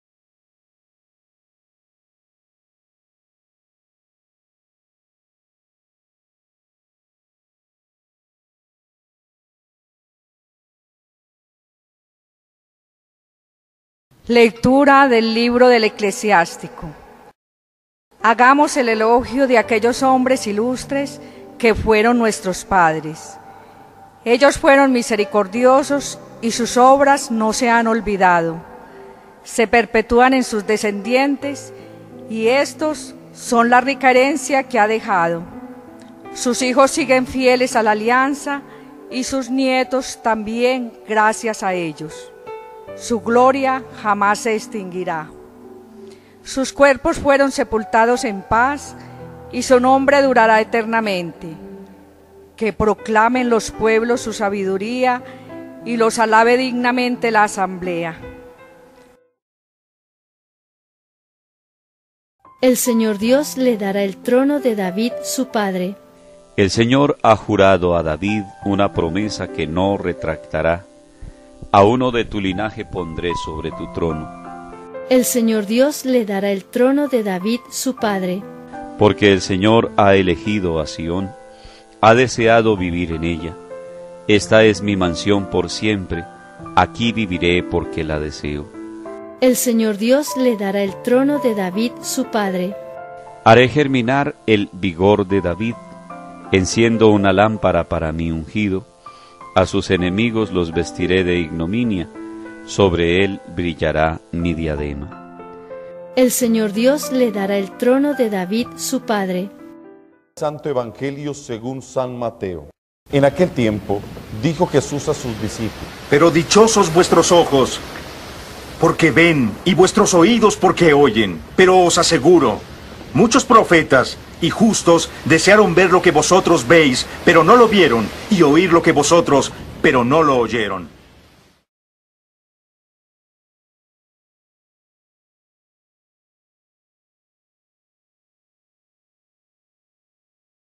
Evangelio del día lecturas jueves 26 de julio de 2018 Fiesta San Joaquín y Santa Ana
LECTURAS Y EVANGELIO PROPIOS DE LA FIESTA SANTOS JOAQUÍN Y ANA PADRES DE LA SANTÍSIMA VIRGEN MARÍA Primera Lectura: Del libro del Eclesiástico (Sirácides) 44,1.10-15 ; Salmo 131 R/. El Señor Dios le ha dado el trono de David, su padre Del santo Evangelio según san Mateo 13,16-17 JUEVES JULIO 26 DE 2018